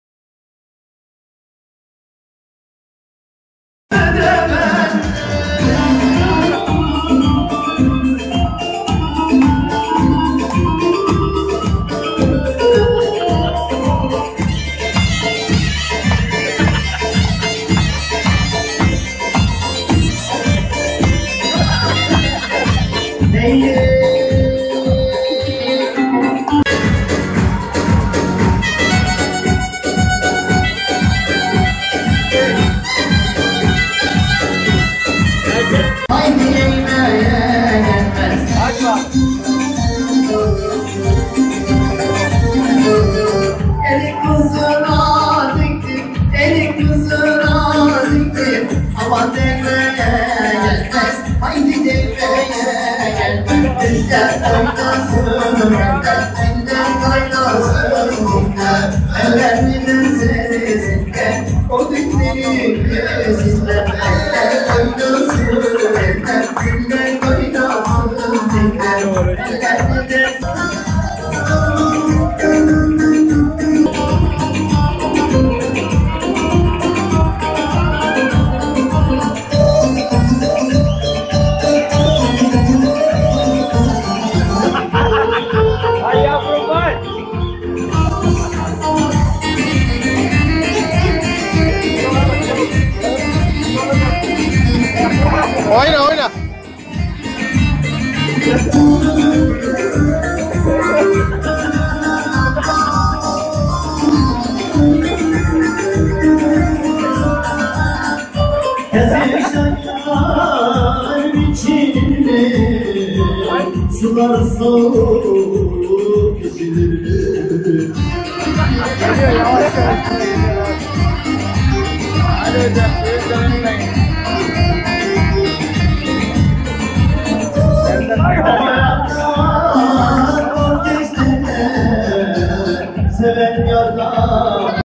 Bisiklete Binemeyen Çocuğun O Günkü Hali
Bugün bisiklete binemeyen çocuğun akşam düğündeki enerji patlaması böyledir:)